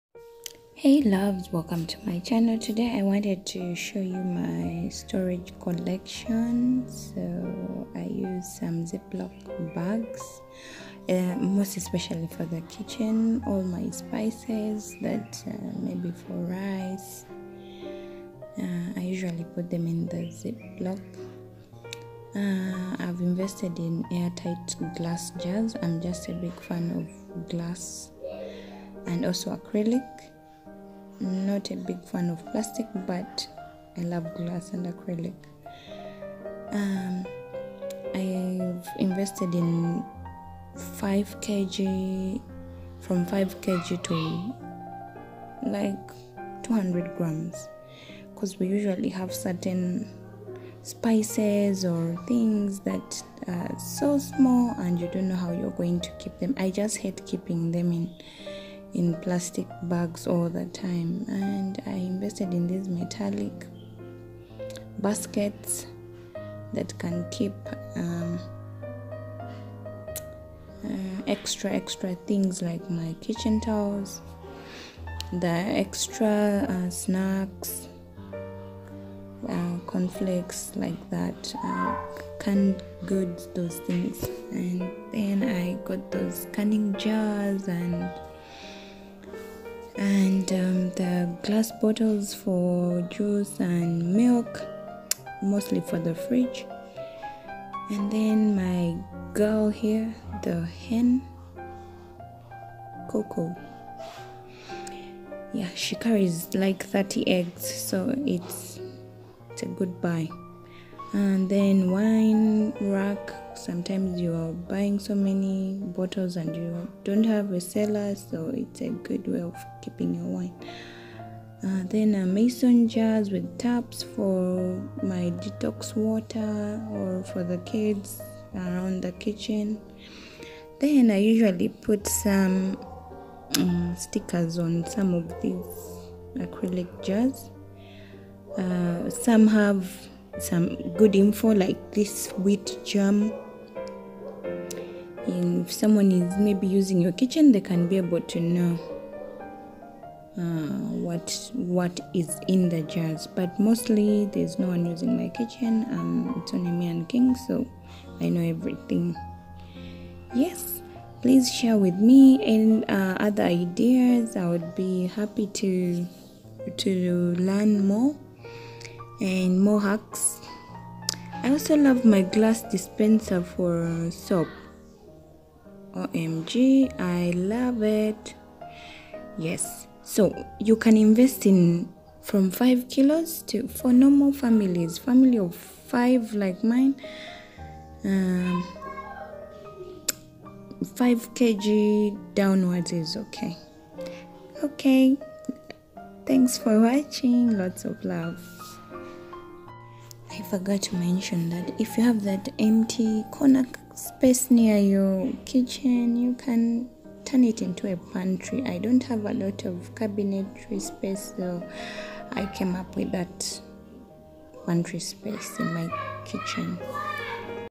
Kitchen Storage Jars,How to organize your small space
What's your favorite storage material😉
What is stopping you from being organized 🤔
An organized space helps you find and work faster in the kitchen. Also it gives a good mood to whoever is in the kitchen .Being organized is a sign of self respect. By keeping organized, you will save time looking for things and will have more time to work on important tasks.
HAPPY ORGANIZING 😍🤗🤩